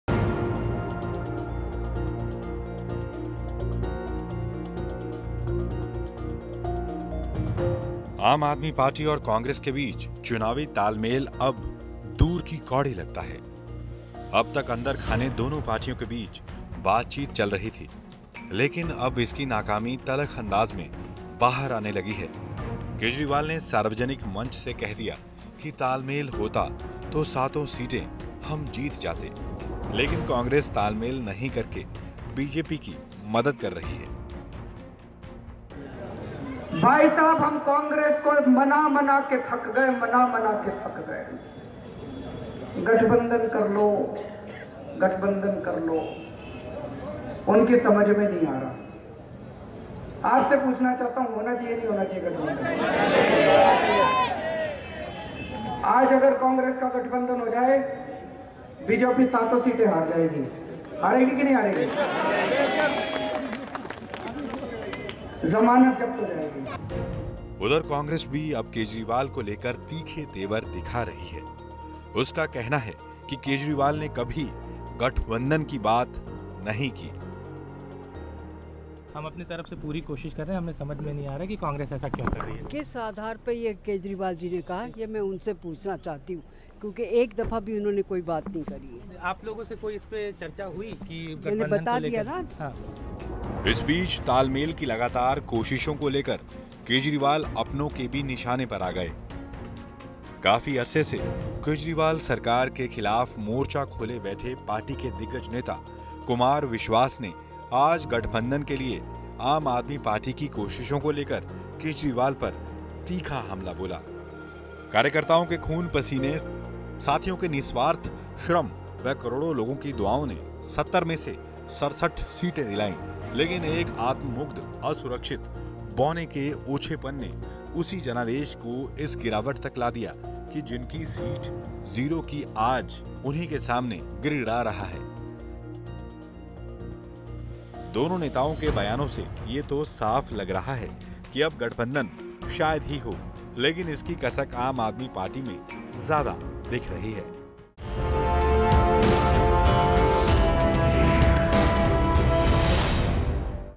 कांग्रेस और आम आदमी पार्टी के बीच गठबंधन को लेकर अब भरोसे की जगह तकरार... | Dilli Tak
कांग्रेस और आम आदमी पार्टी के बीच गठबंधन को लेकर अब भरोसे की जगह तकरार ने ले ली है। आम आदमी पार्टी ने अब खुले तौर पर कांग्रेस पर हमले बोलने शुरू कर दिये हैं. उधर कांग्रेस ने ये कहकर चौंका दिया कि खुद केजरीवाल ने कभी गठबंधन की बात नही की थी।